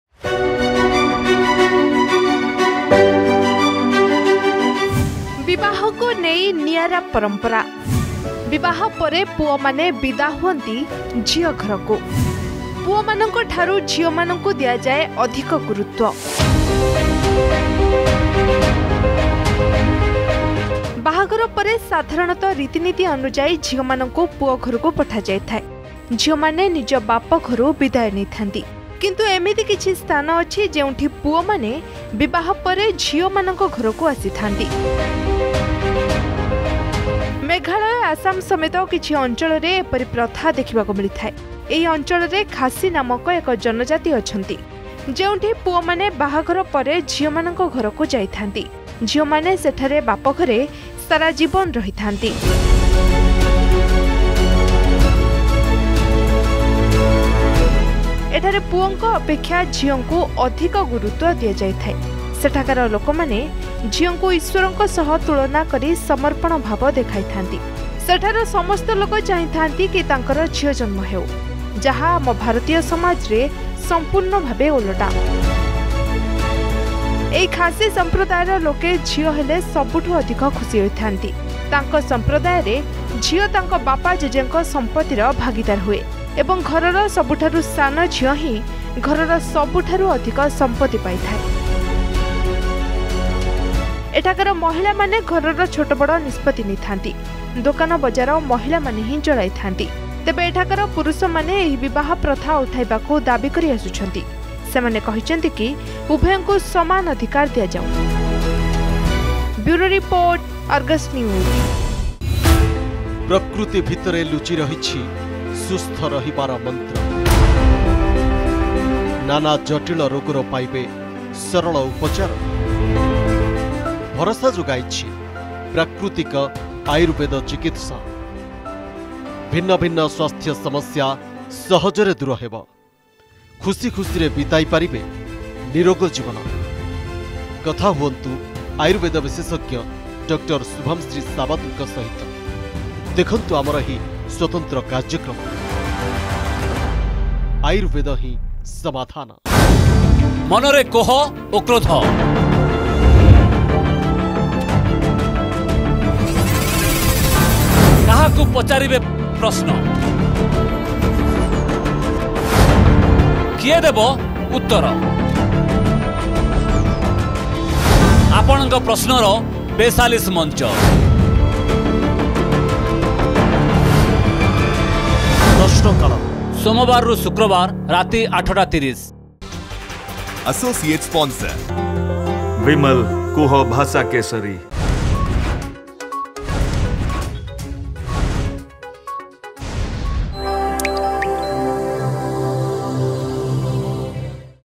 ବିବାହକୁ ନେଇ ନିଆରା ପରମ୍ପରା
ବିବାହ ପରେ ପୁଅମାନେ ବିଦା ହୁଅନ୍ତି ଝିଅ ଘରକୁ
ପୁଅମାନଙ୍କ ଠାରୁ ଝିଅମାନଙ୍କୁ ଦିଆଯାଏ ଅଧିକ ଗୁରୁତ୍ବ
ବାହାଘର ପରେ ସାଧାରଣତଃ ରାତିନୀତି ଅନୁଯାୟୀ ଝିଅମାନଙ୍କୁ ପୁଅଘରକୁ ପଠାଯାଇଥାଏ ।  ଝିଅମାନେ ନିଜ ବାପଘରୁ ବିଦାୟ ନେଇଥାନ୍ତି । କିନ୍ତୁ ଏମିତି କିଛି ସ୍ଥାନ ଅଛି ଯେଉଁଠି ପୁଅମାନେ ବିବାହ ପରେ ଝିଅ ଘରକୁ ଆସିଥାନ୍ତି । ମେଘାଳୟ ଆସାମ ସମେତ କିଛି ଅଞ୍ଚଳରେ ଏପରି ପ୍ରଥା ଦେଖିବାକୁ ମିଳିଥାଏ ।